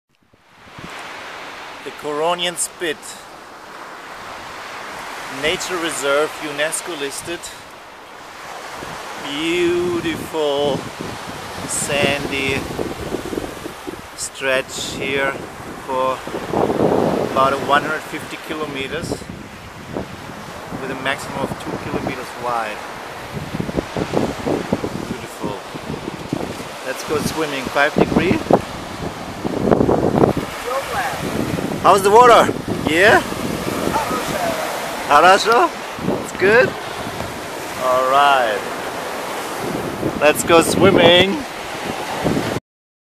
Kaliningrad Beach – Curonian Spit
The Curonian Spit is a wonderful nature place where you will find absolutely clean water & beach. Only 70km away, in Polish Gdansk, the water is full of jelly fish.